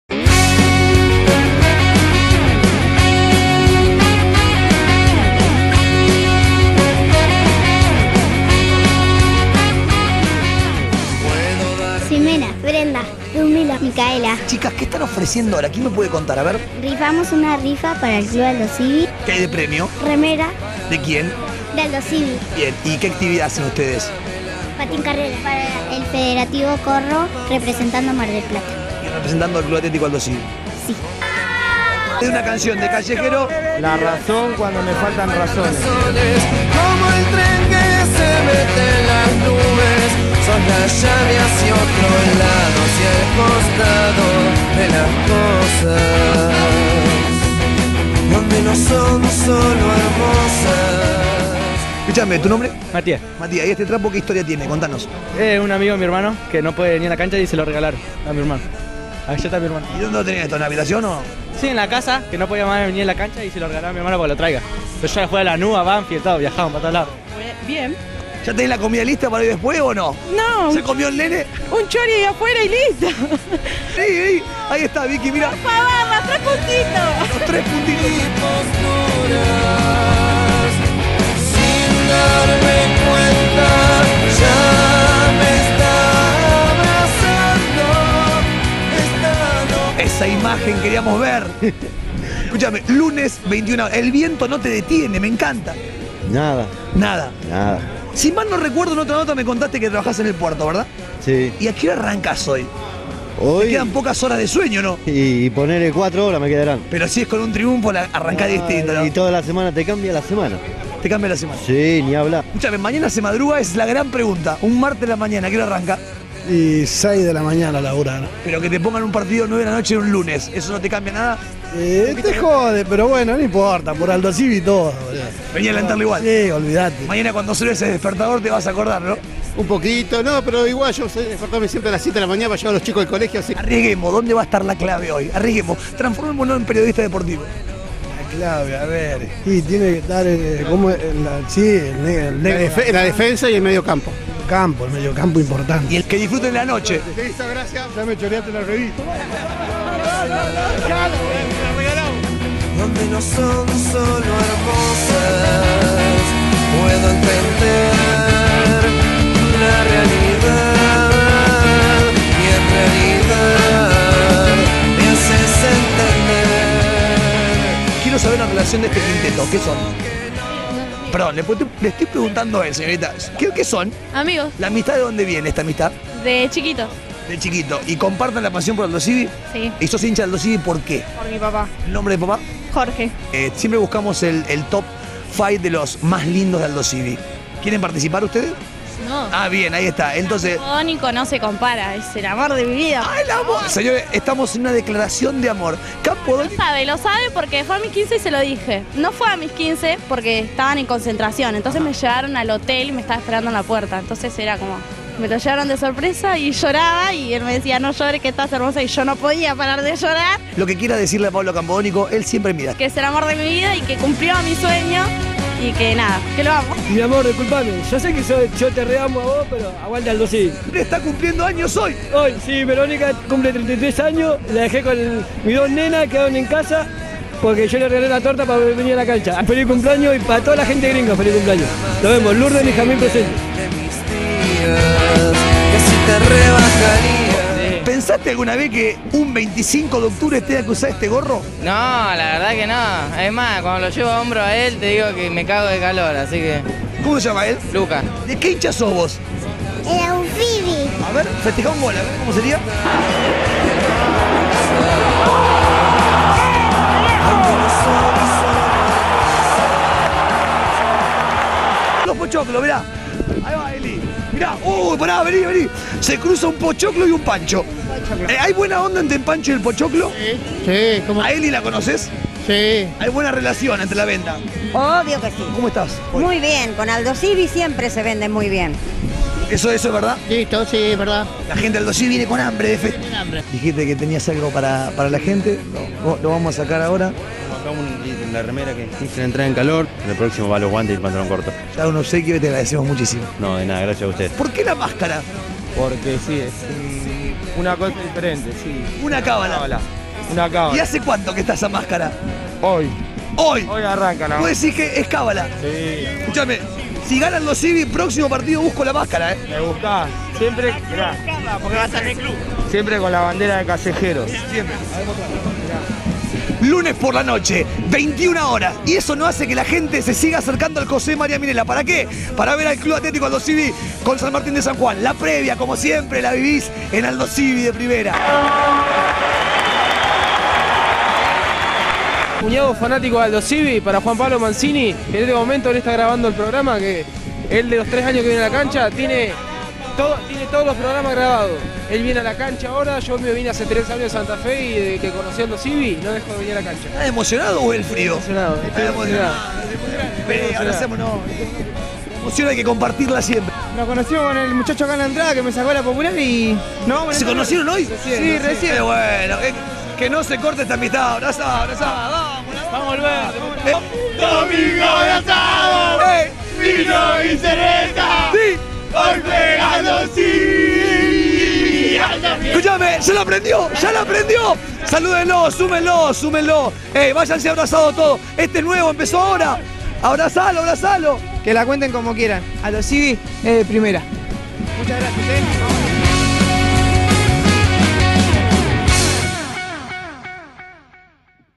Que una derrota no empañe tanta alegría, otra previa de Aldosivi de Primera
La clásica previa de Juan Pablo "Corcho" Bottiglieri, el calor y el color del pueblo de Aldosivi que fue el lunes por la noche a la cancha para alentar al tiburón frente al santo sanjuanino.